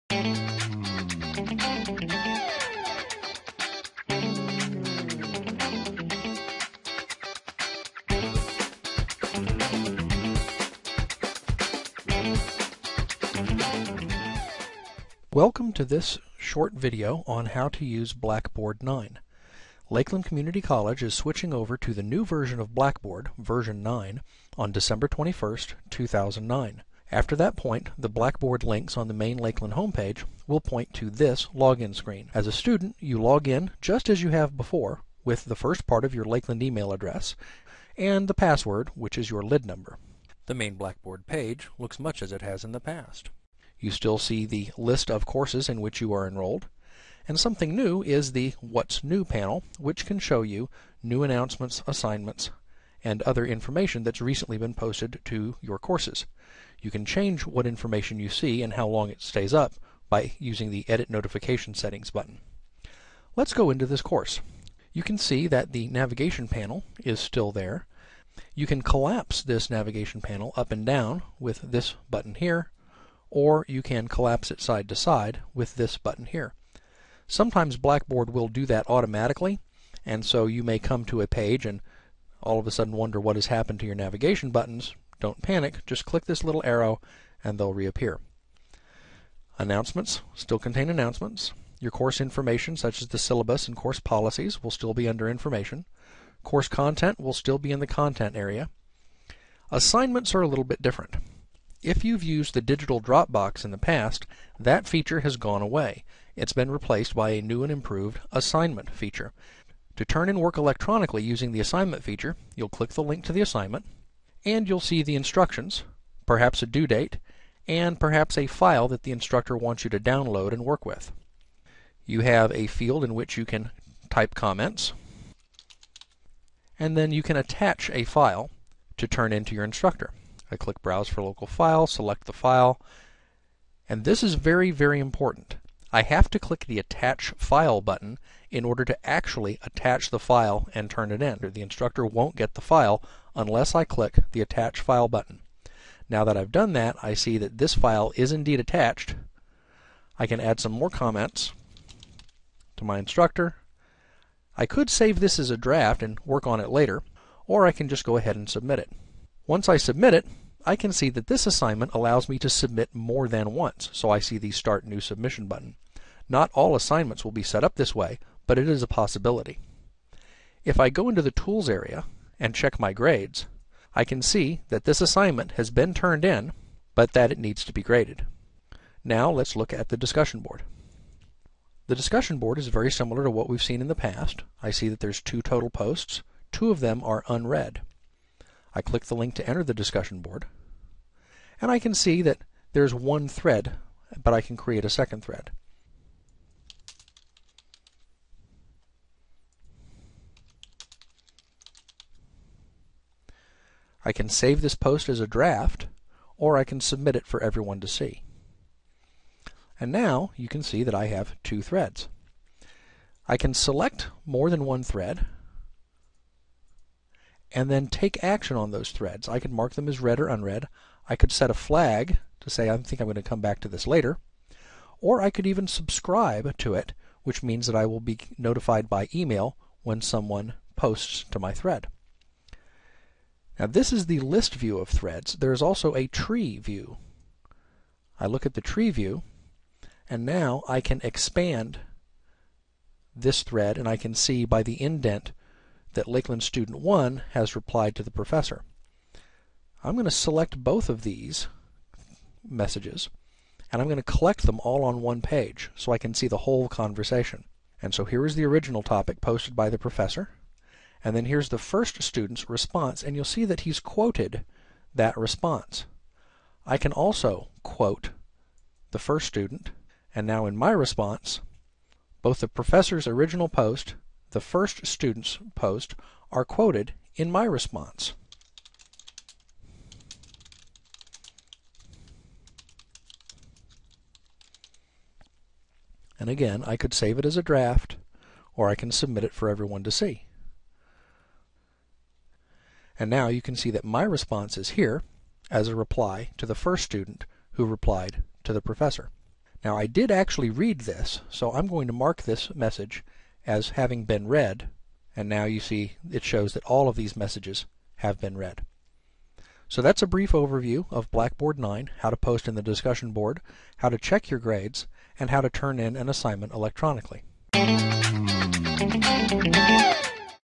Blackboard 9.0 Student Overview
This video gives a student's view of basic operations in Blackbord version 9.0.  Note that Service Pack 3 removes the need to click the Attach File button a second time.